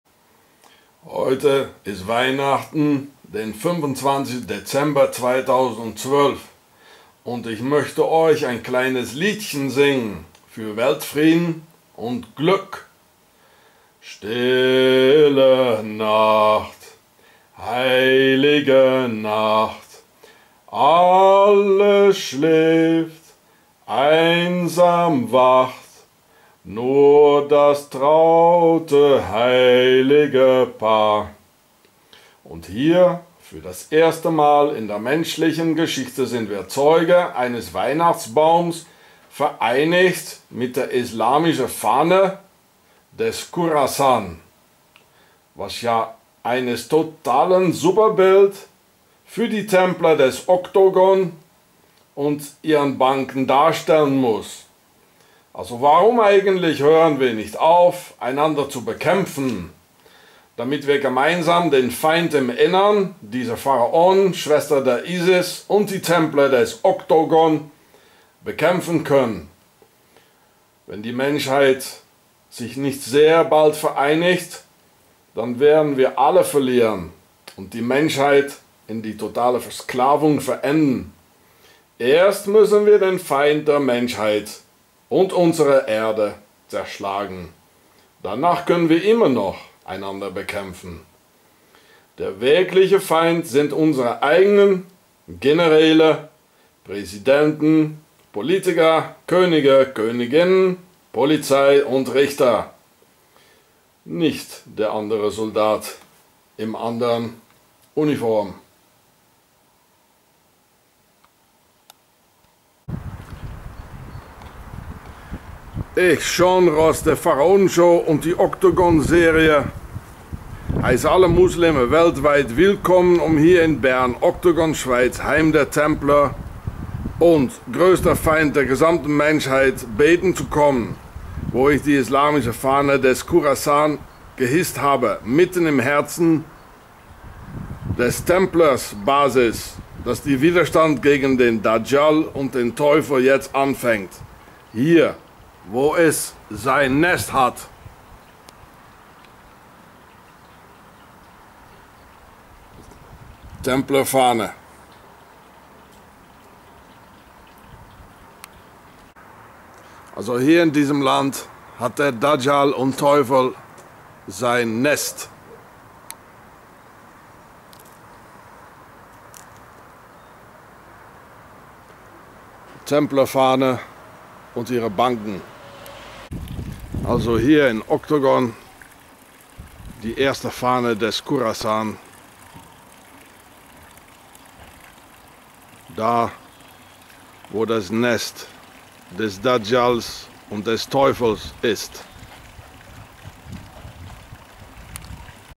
Weltfrieden: Khourassan, Weihnachten, Vereinigte Welt vs.Templer von Octogon Nest des Dajjal Teufel
Wenn die gesamte Menschheit sich nicht sehr rasch vereinigt um sich die Mächte der Finsternis zu wehr zu setzen, dann werden wir alle in die totalen Versklavung verenden; der Feind im Inneren ist sehr real, und die Pharaonen, Schwester der Isis und die Templer des Octogon und ihre Banken sind noch immer da und besetzen alle Schlüsselpositionen der Gesellschaft.